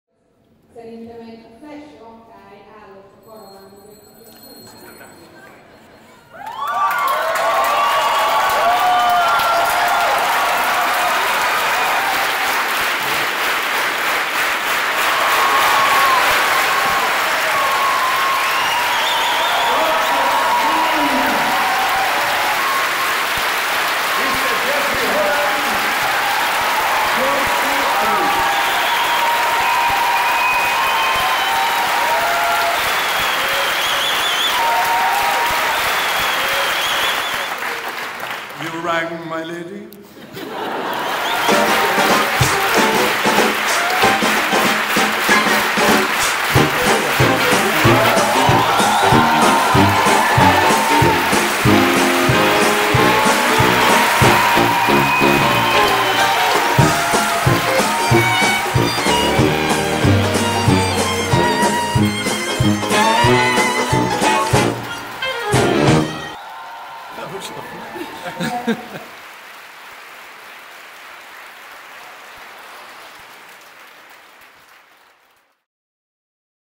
James Twelvetrees at Budapest
Jeffrey Holland better known as James Twelvetrees enters the stage at the 30 th anniversary show of You Rang Mylord in Budapest 2018.